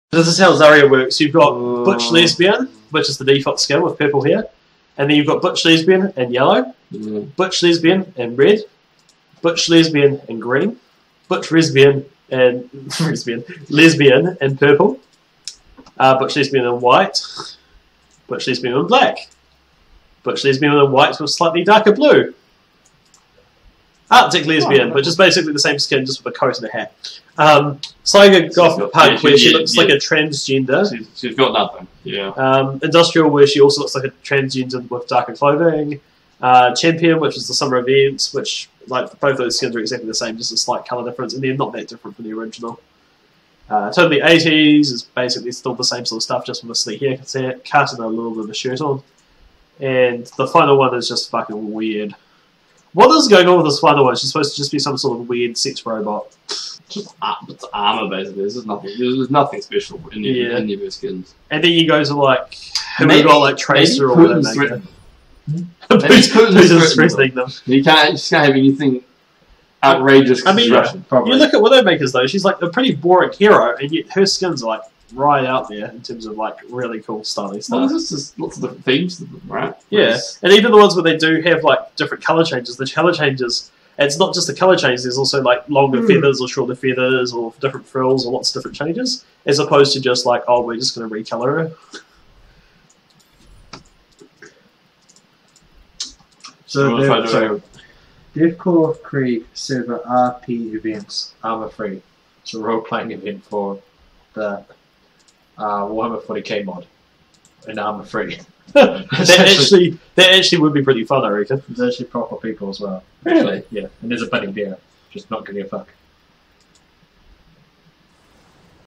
The problem with Overwatch Srow explains Zarya boring skins
My biggest gripe with the skins in overwatch is that some of the heroes really lack interesting skins ie Zarya. Basically, us having an in-between game gripe about the loot boxes unlock system.

It seems like some heroes get all the design and skin change effort and other get near none

Perhaps new skins are made in alphabetical order and they just get tired?